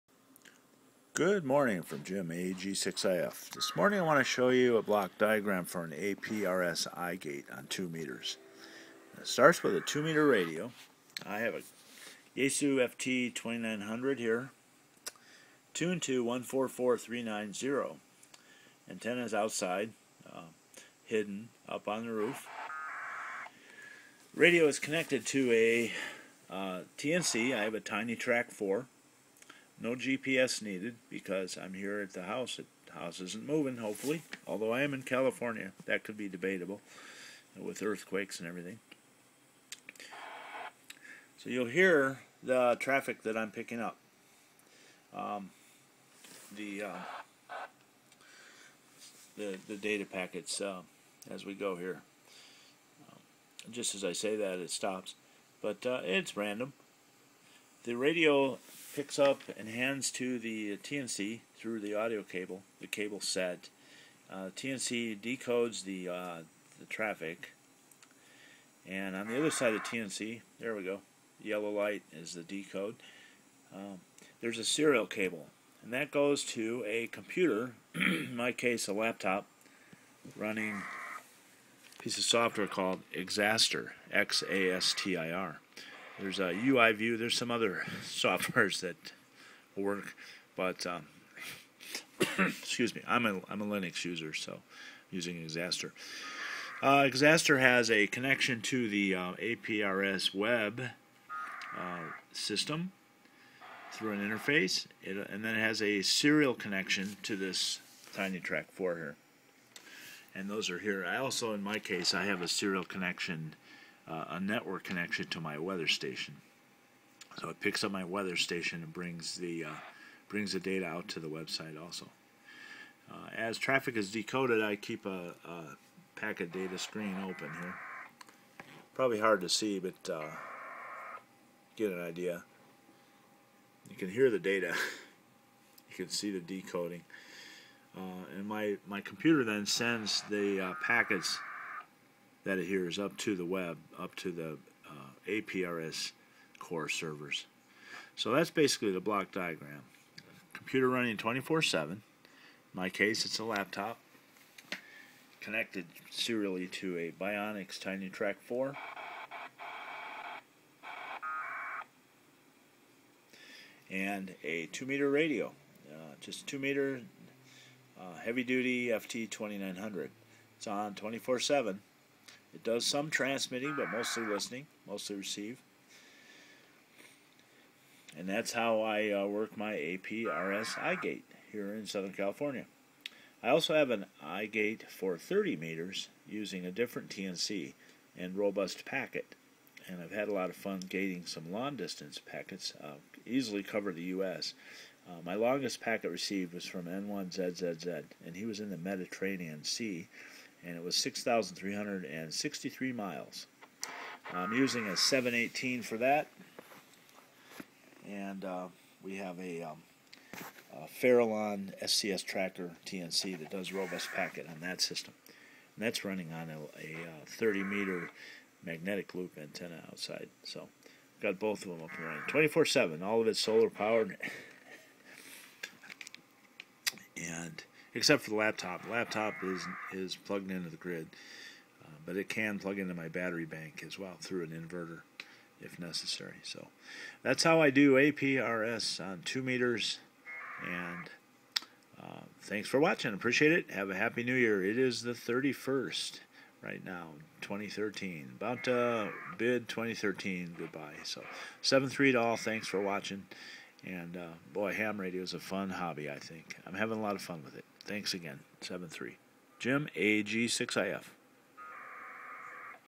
APRS 2 meter iGate block diagram 144.390
A quick video to show the block diagram connectivity on my 2m iGate for APRS, serving parts of Southern California --Temecula.

A 2m radio (ft-2900) - Cables - Byonics TinyTrak4 - Cables - Laptop running application called Xastir - connected to the Cloud Internet to APRS-Core servers.

Pathway is as follows:
Radio hears APRS data stream on 144.390, passess signal to TinyTrak4 which decodes it, and sends it to the serial port on the laptop which then passes the data to the APRS core servers connected to the Cloud (Internet.)

For the reverse, any data originating from the cloud, which has a destination here in Temecula, and my iGate has heard the local station within the last 60 minutes, the packet flow is as follows:

Data is sent from the Cloud to all logged in APRS laptops/computers.  If the station is local to my station, the Xastir software sends the packet to the TinyTrak4 TNC which sends a command to the radio which causes the packet to transmit over the airwaves via RF.